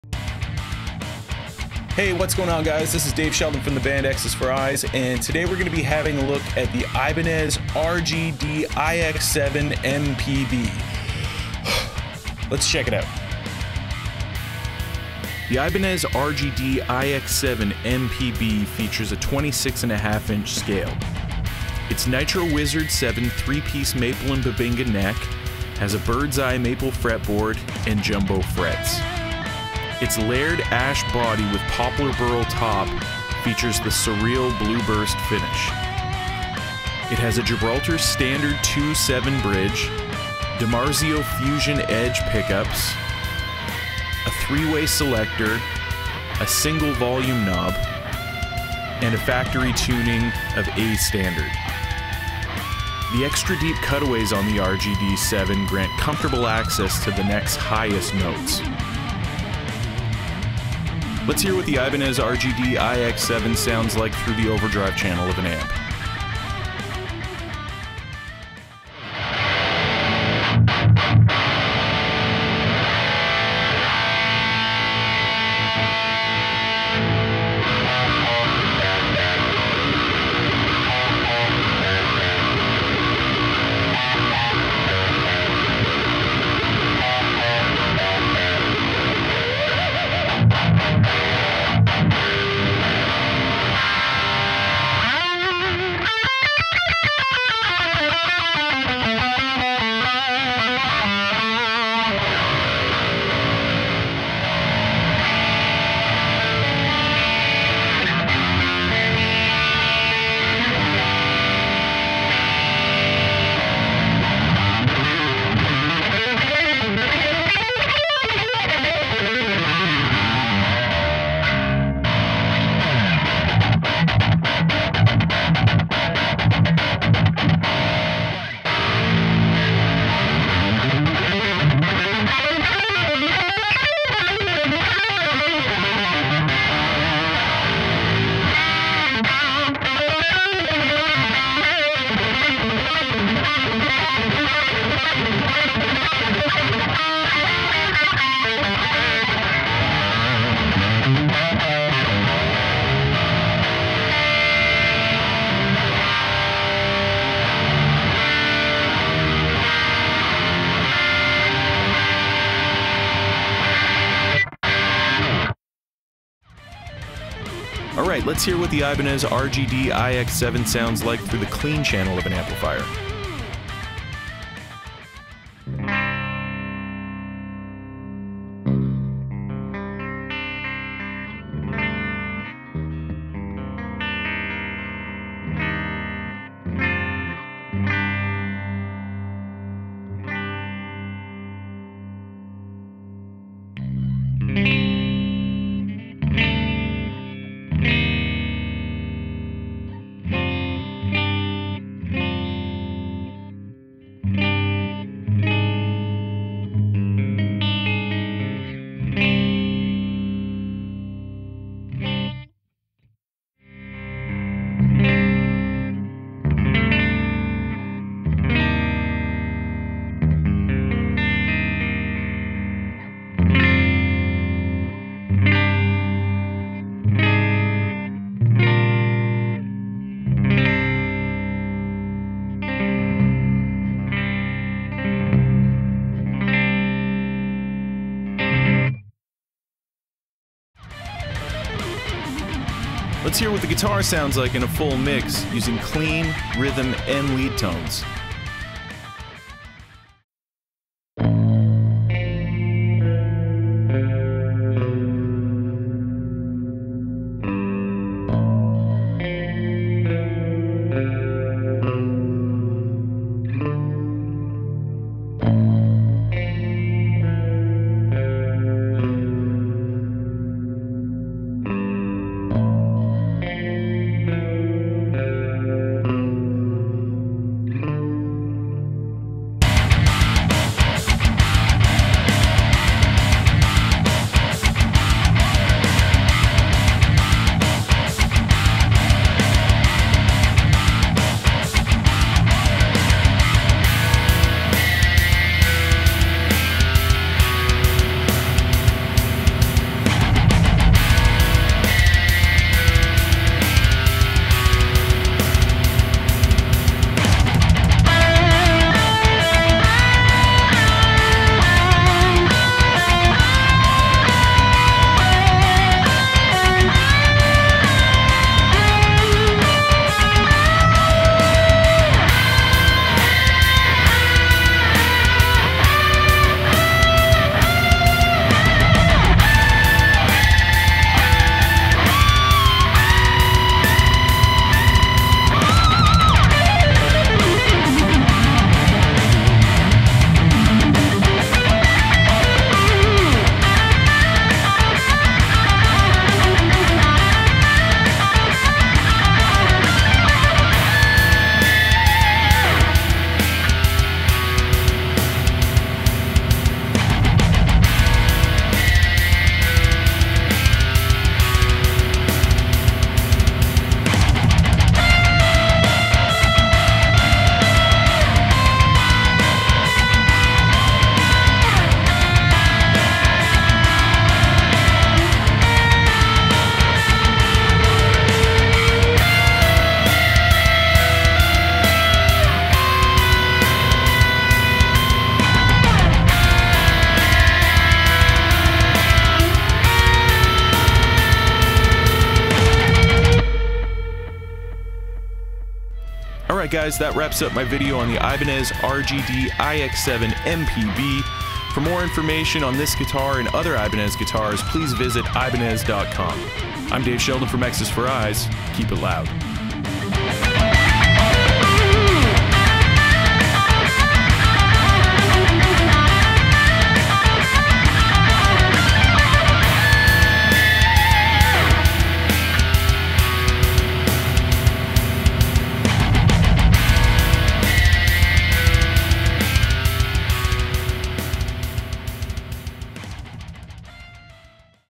Ibanez RGDIX7MPB - A Full In-Depth Demonstration
Dave Sheldon of EXES FOR EYES goes through the ins and outs of the 2016 Ibanez RGDIX7MPB.  This video covers aesthetics, specs, and sounds (clean, dirty, and full mix).  For even more information on the RGDIX7MPB, please

Special thanks to Mike Marino and the rest of the staff at
EFKAY MUSIC in Quebec!